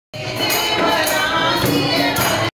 Shawn made my video first time 05102008
Shawn made my video first time 05102008. He took camera from me and trying to take picture but made video